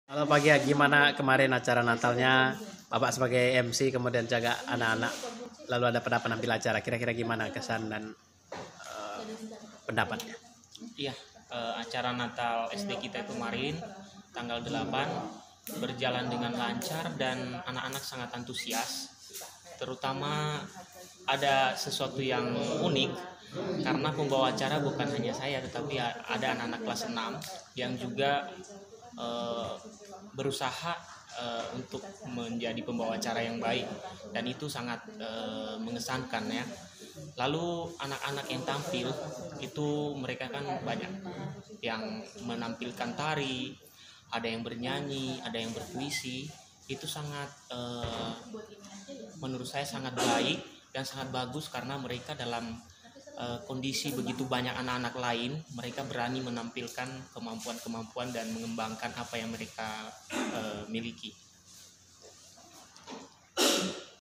Natal SD. Tarsisius Vireta Sukses. Ini Kata Mr. Gea
Tanggapan terhadap perayaan Natal SD. Tarsisius Vireta

Kunjungi juga YT, FB, Twitter, IG De Gea's Official